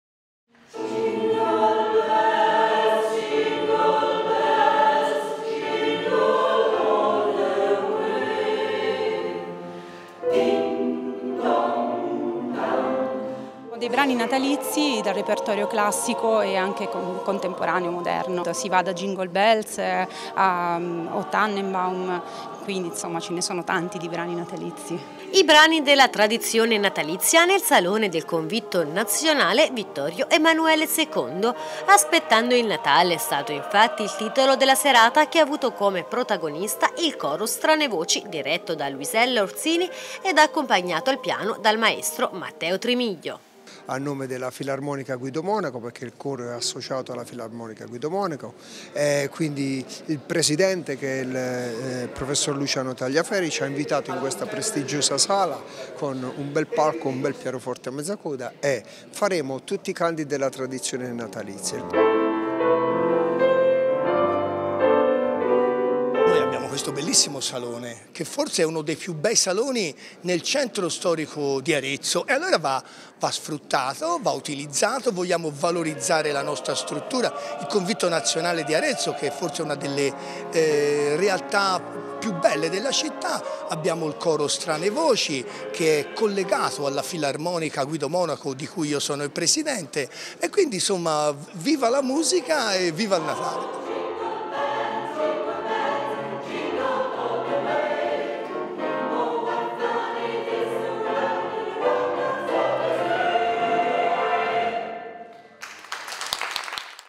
Il Coro Stranevoci al Convitto per il concerto "Aspettando il Natale"
I  brani della tradizione natalizia  nel salone del Convitto Nazionale Vittorio Emanuele II.
“Aspettando il Natale” è stato il titolo della serata, che ha avuto come protagonista il Coro Strane Voci.